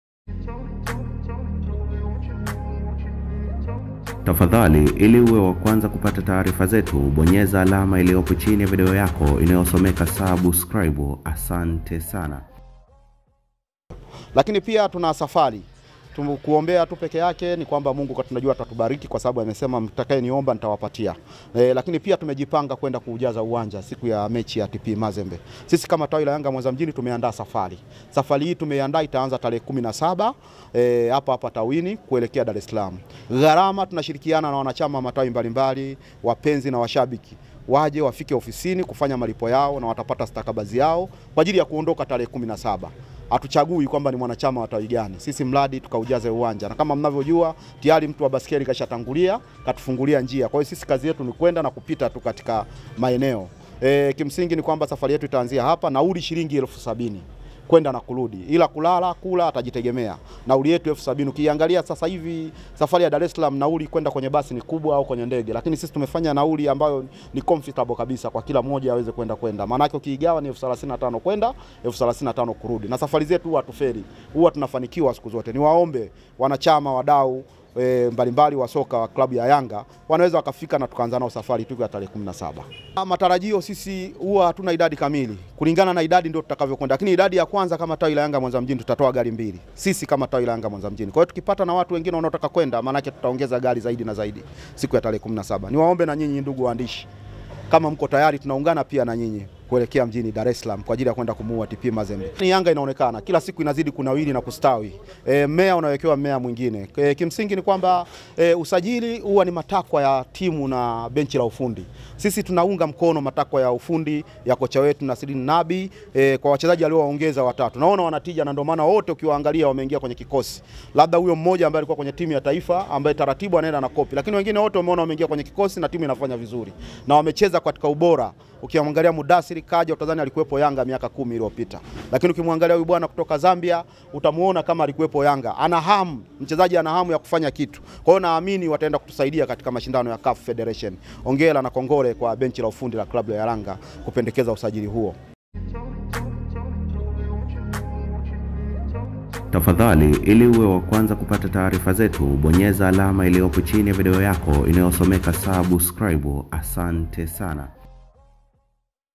YANGA MWANZA KUONGEZA HAMASA TP MAZEMBE
Licha ya Klabu ya soka ya Yanga kupoteza mchezo wake wa kwanza hatua ha Makundi dhidi ya Us Monastry Mashabiki wa klabu hiyo Mwanza wamepanga kusafiri kwenda Dar kwaajili ya kuipa nguvu klabu yao kuelekea mchezo wa February 19 dhidi ya TP Mazembe utakaochezwa uwanja wa Mkapa jijini Dar.